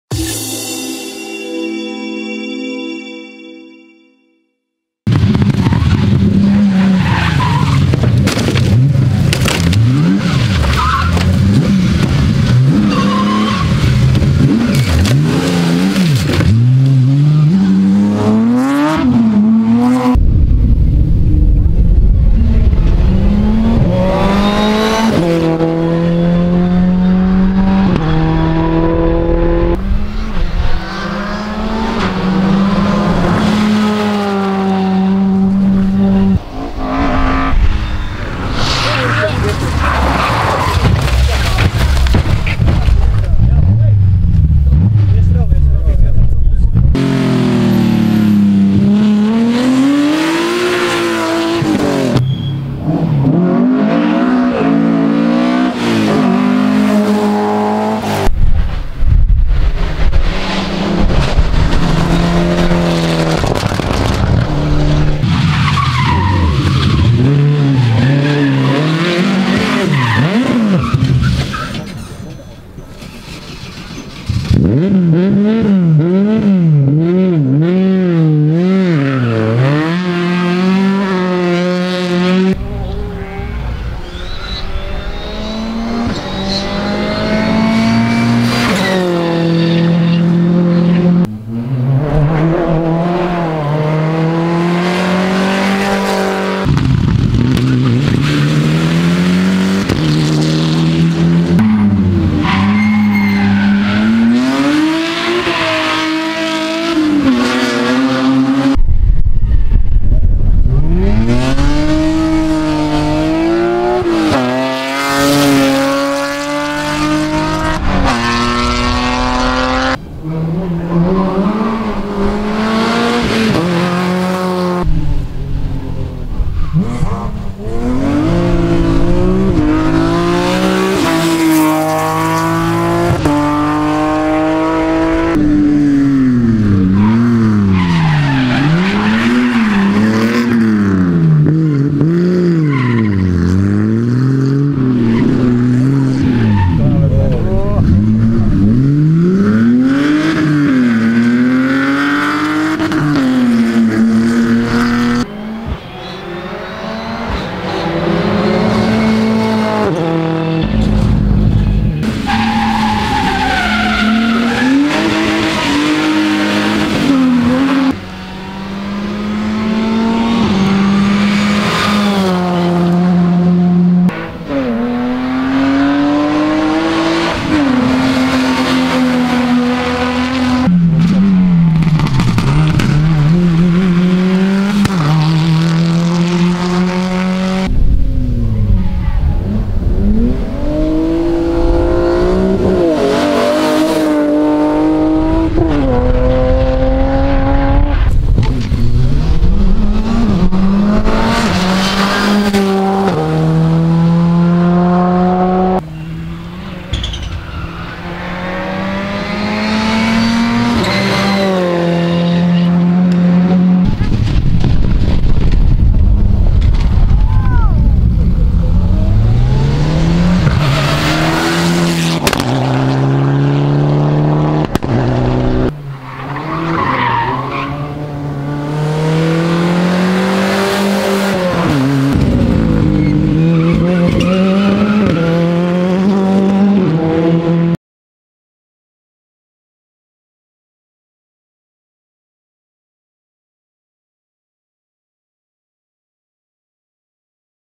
Rally van Staden 2016 | Show - Crash - Mistakes
Onze beelden van de Rally van Staden 2016.

Visit and like our FaceBook page for HD rally pictures